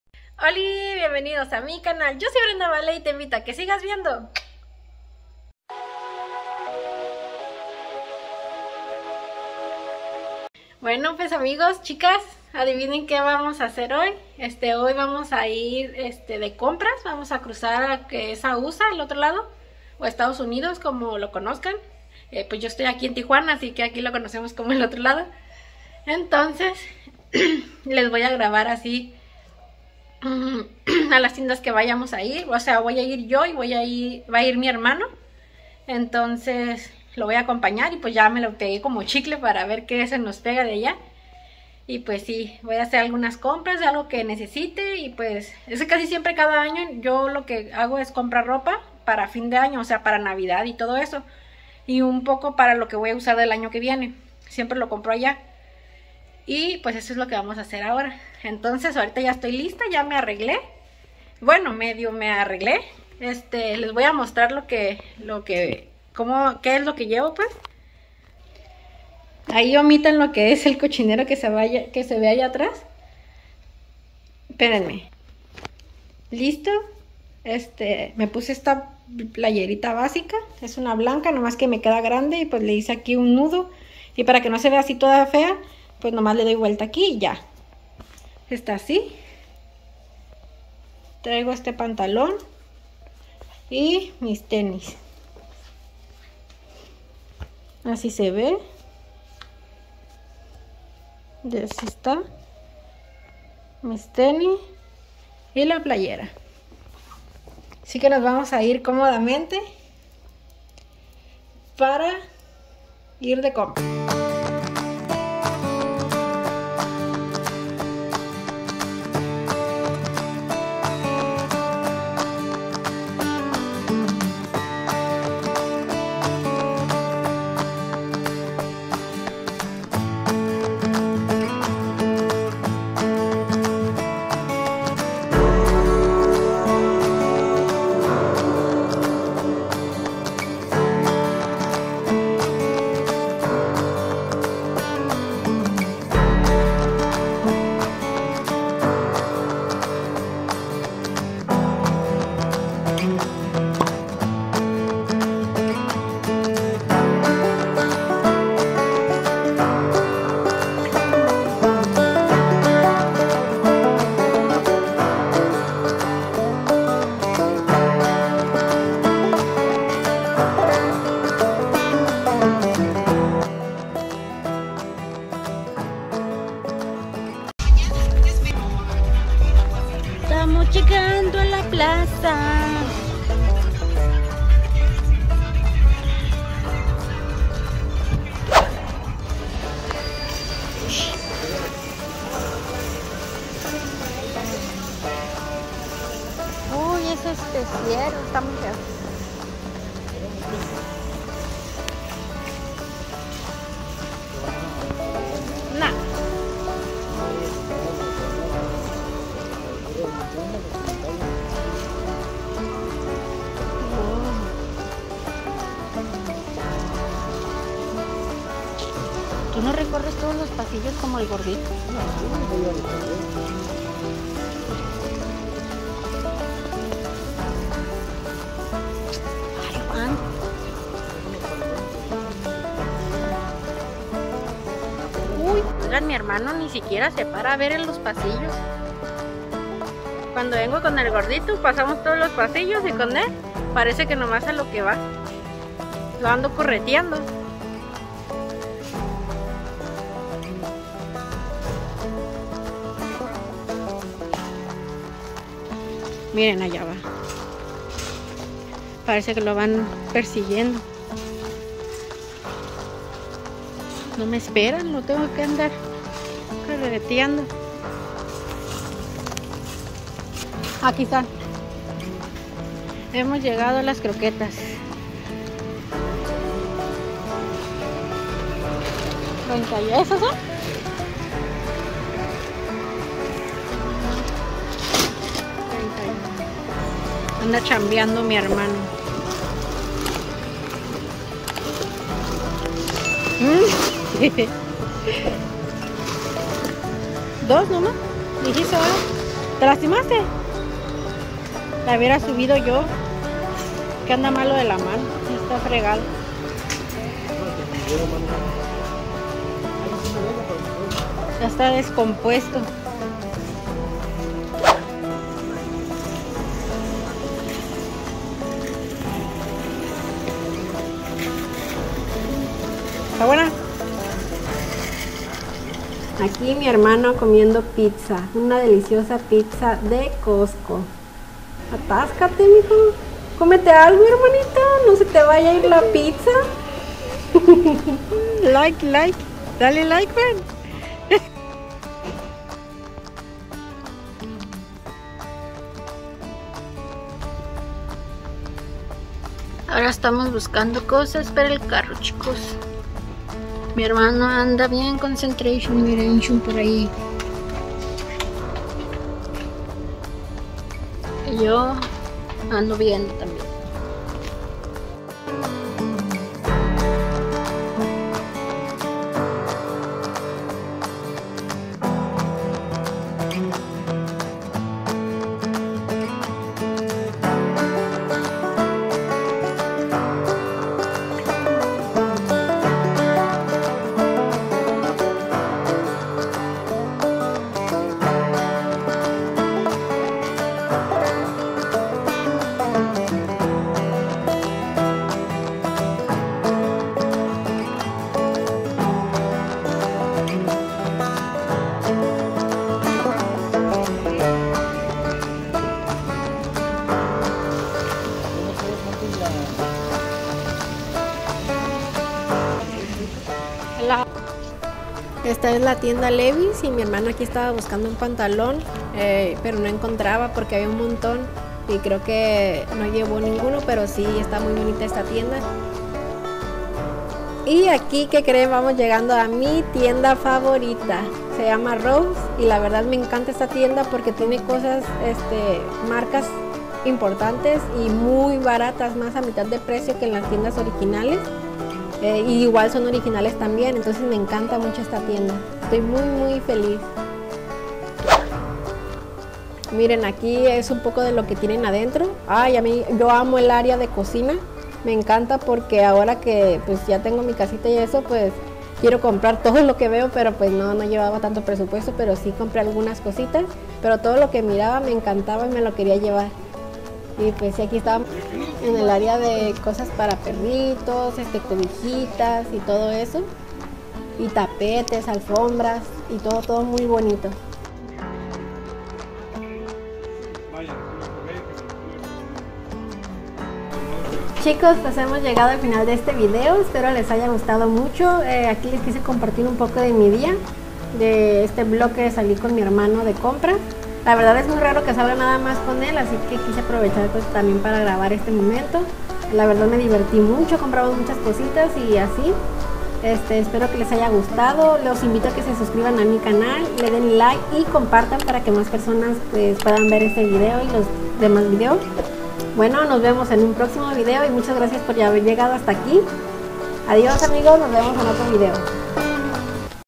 ✅️De compras tienda ROSS! y COSTCO con mi hermano en USA.!vlog
Hola, bienvenidos a mi canal, esta ves les comparto un vlog de compras con mi hermano fuimos a usa a comprar cositas de fin de año🙂
Espero les guste mucho y se queden hasta el final

🔔Activar la campanita de notificaciones
y poder ser más personitas en esta nueva familia.

brendavalee25